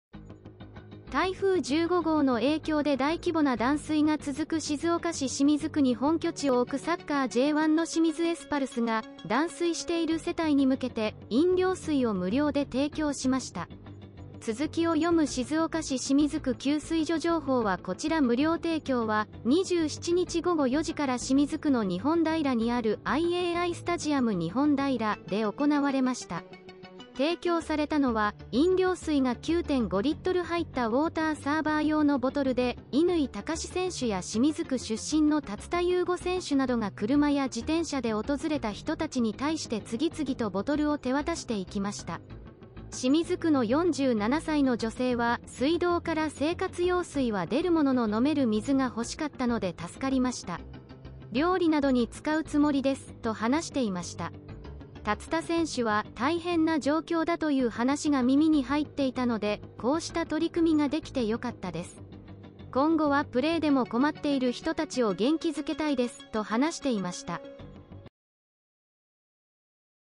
「ニュース」台風15号で大規模断水 清水エスパルスが無料で飲料水を提供
台風15号の影響で大規模な断水が続く静岡市清水区に本拠地を置くサッカー・J1の清水エスパルスが、断水している世帯に向けて、飲料水を無料で提供しました。

続きを読む静岡市清水区給水所情報はこちら無料提供は、27日午後4時から清水区の日本平にある「IAIスタジアム日本平」で行われました。提供されたのは、飲料水が9.5リットル入ったウォーターサーバー用のボトルで、乾貴士選手や清水区出身の立田悠悟選手などが車や自転車で訪れた人たちに対して次々とボトルを手渡していきました。清水区の47歳の女性は「水道から生活用水は出るものの飲める水がほしかったので助かりました。料理などに使うつもりです」と話していました。

立田選手は「大変な状況だという話が耳に入っていたので、こうした取り組みができてよかったです。今後はプレーでも困っている人たちを元気づけたいです」と話していました。